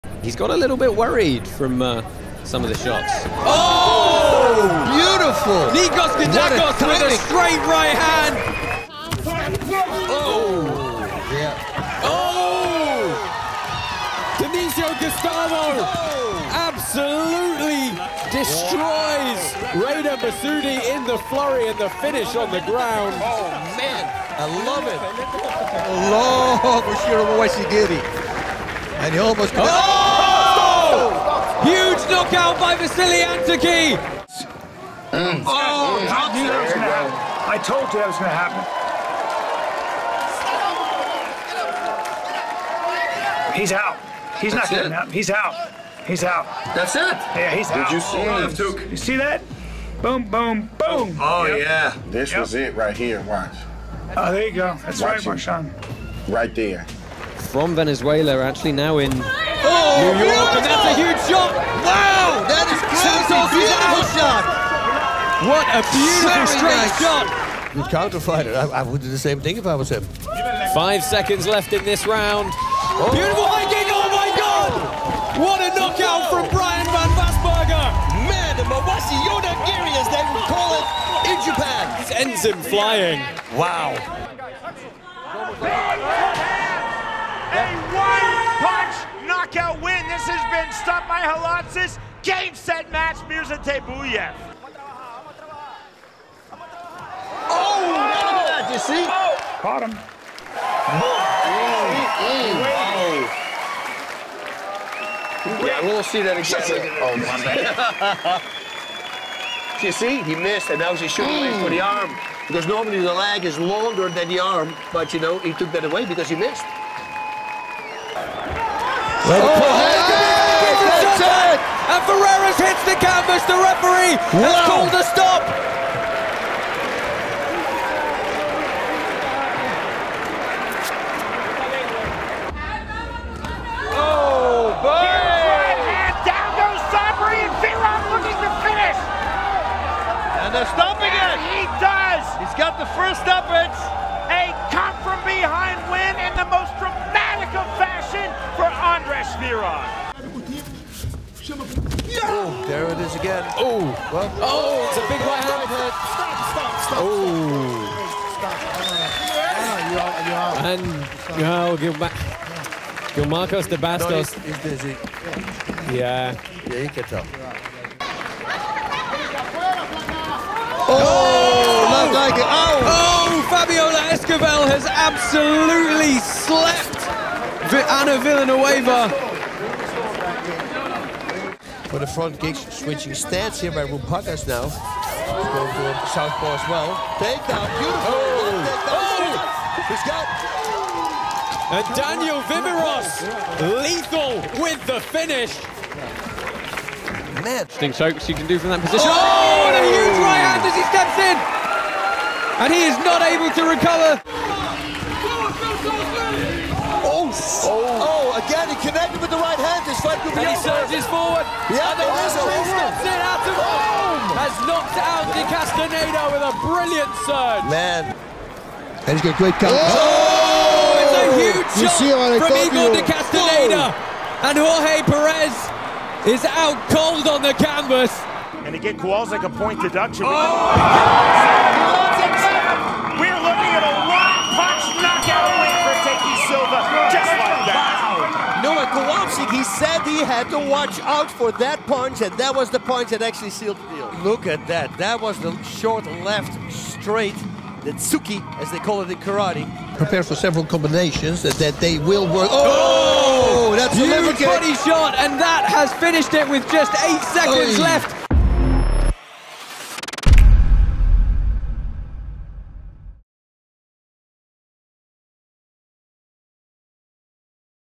CRAZIEST KNOCKOUTS IN KARATE COMBAT
Straight up the best knockouts in Karate Combat history with more to come in Season 4 coming Sprint 2022.